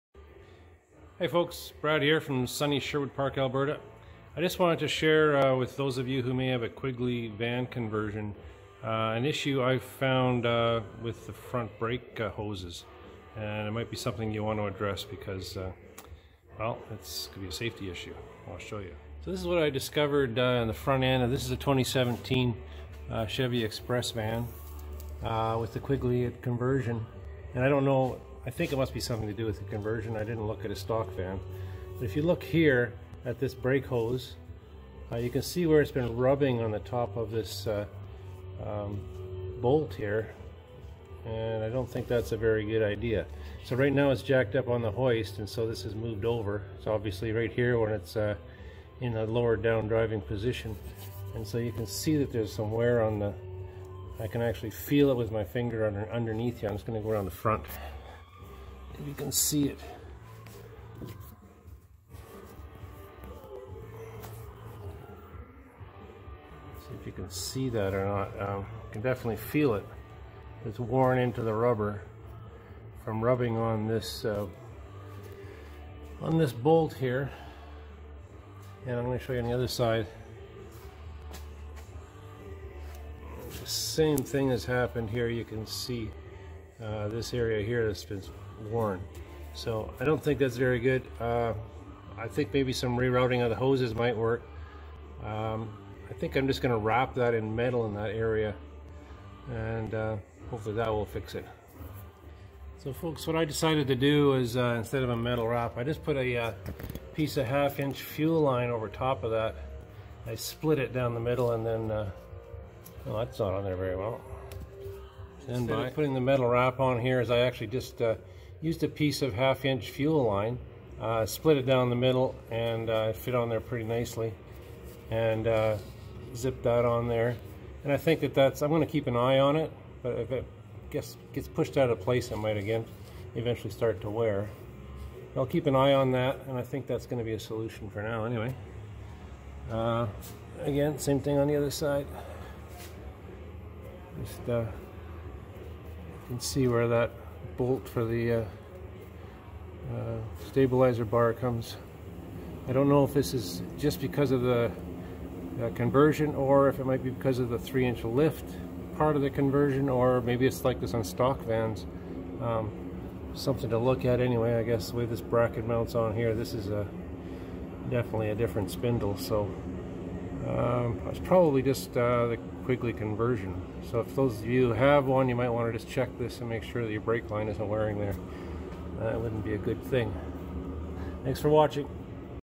Quigley Express Van 4x4 Conversion Potential Problem
If you own a Quigley converted Express or Savana van you should check this potential problem soon!!!